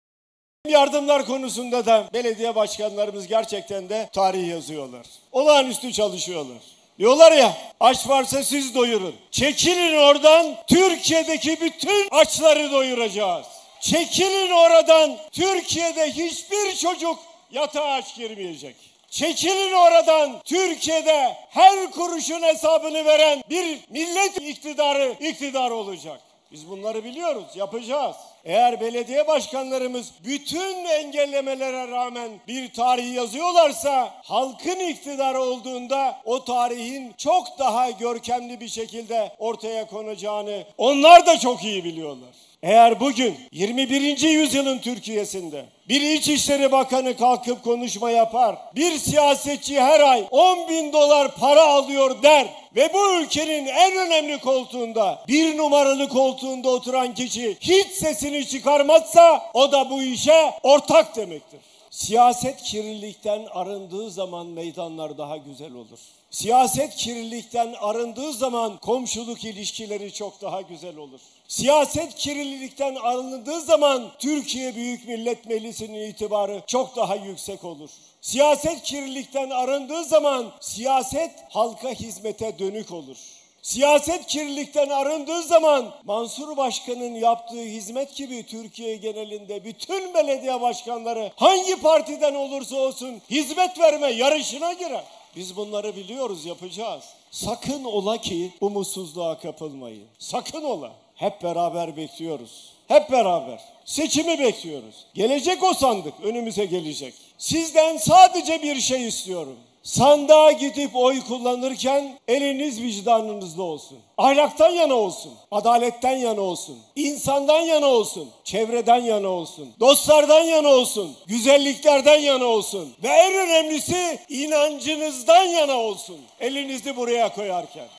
Kılıçdaroğlu'ndan Erdoğan'a yanıt: Aradan çekilin, tüm açları doyuralım
CHP Genel Başkanı Kemal Kılıçdaroğlu, Cumhurbaşkanı ve AKP Genel Başkanı Recep Tayyip Erdoğan'ın muhalefete yönelik "Millet açsa siz doyurun" sözlerine yanıt verdi. Kılıçdaroğlu, "Çekilin aradan Türkiye'de hiçbir çocuk yatağa aç girmeyecek. Çekilin aradan Türkiye'de her kuruşun hesabını veren bir Millet ittifakı iktidar olacak" dedi.